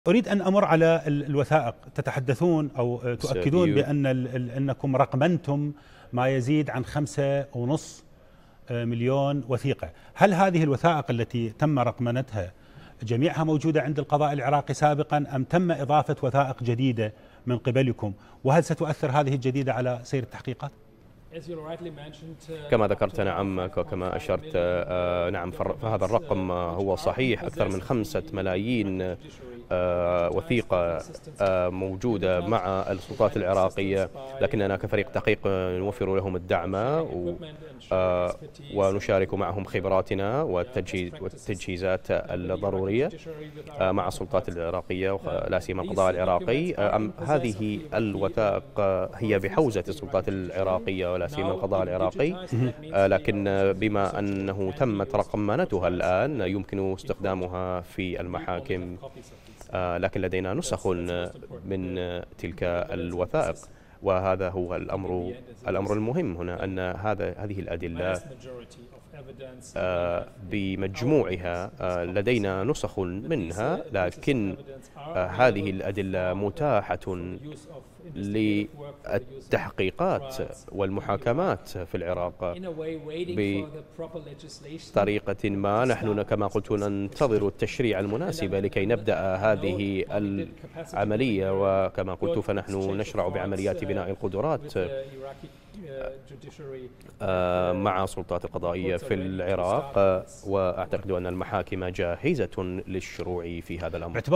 كريستيان ريتشر: هناك أكثر من 5 ملايين وثيقة بحوزة السلطات العراقية تخص تنظيم داعـش الإرهـابي
في ذكرى تحـرير الموصل.. الأمم المتحدة تكشف نتائج تحقيقاتها في جـرائم داعـش الإرهابـي في المحايد مع سعدون ضمد
الضيف: كريستيان ريتشر | المستشارِ الخاصِّ بفريقِ الأممِ المتحدةِ للتحقيقِ بجرائمِ داعش في العراق "يونيتاد"